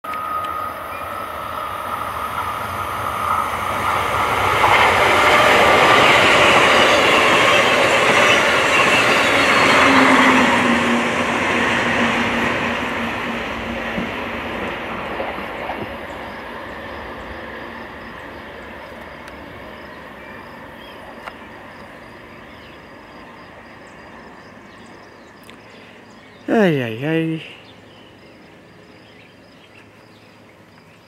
amtrak keystone #655
on this episode of steam enthusiast productions we will... see the amtrak keystone come flying by!